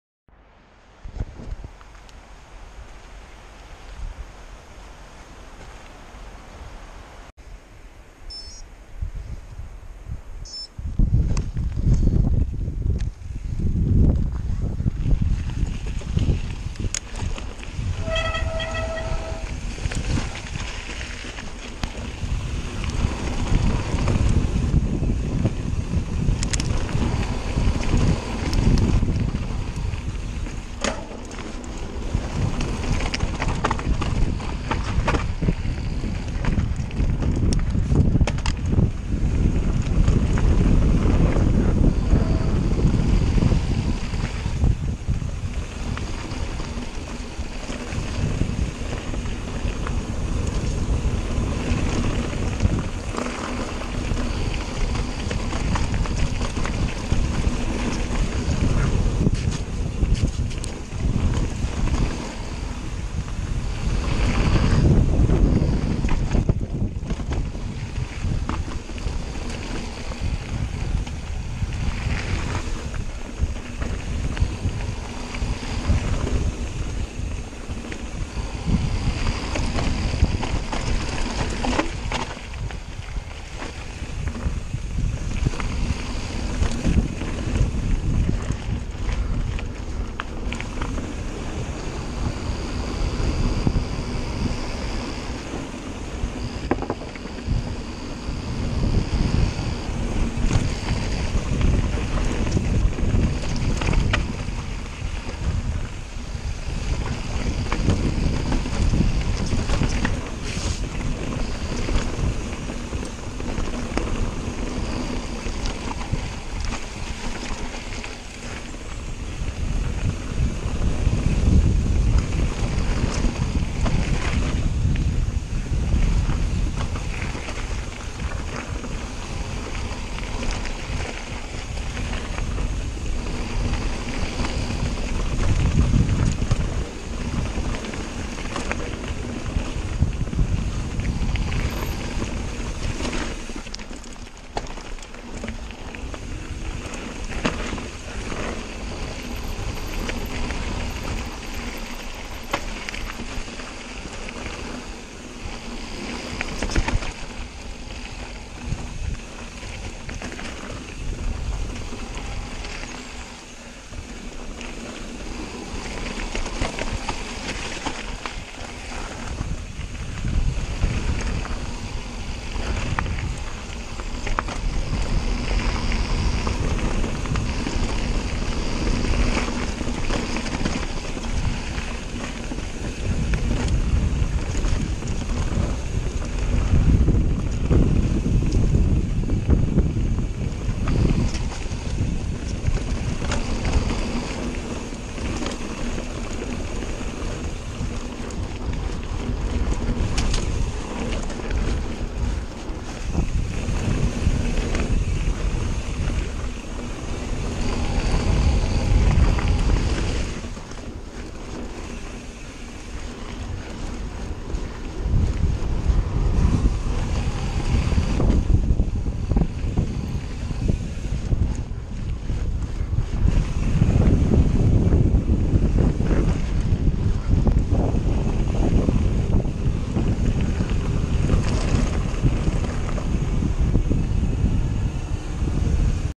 Cotic Flare Down Blue Adder
My 3rd Run down Blue Adder, each one quicker than the last.
Absolutely loving the Flare,